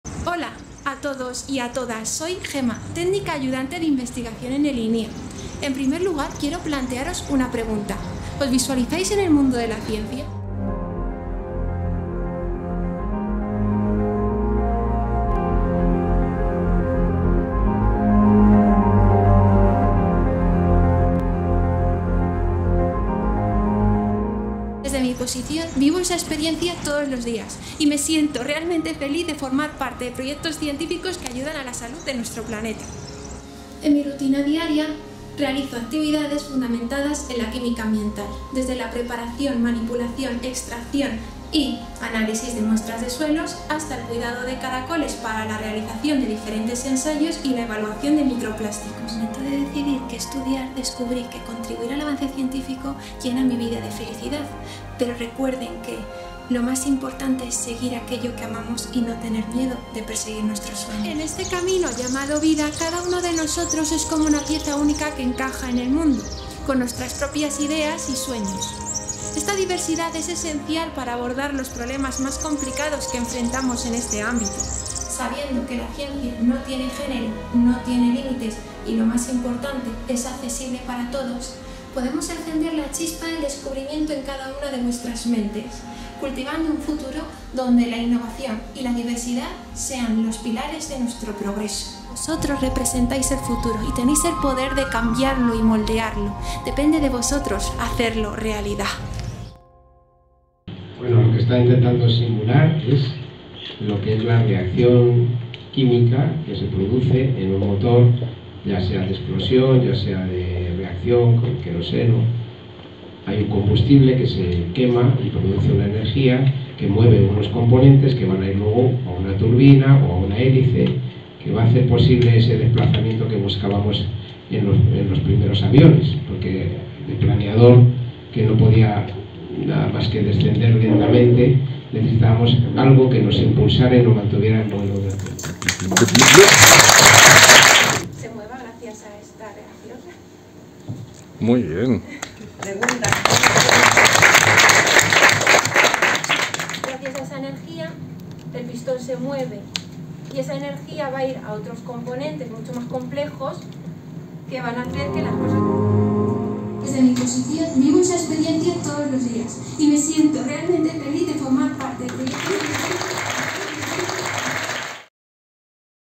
Talleres STEM de Boeing en la Fundación Infante de Orleans en colaboración con el NIA-CSIC
En los talleres de Boeing en la Fundación Infante de Orleans, Gema Sáez describe su papel como técnico de laboratorio en el INIA-CSIC y comparte la magia de las reacciones químicas con los más jóvenes con el objetivo de fomentar en ellos la pasión por la ciencia y la tecnología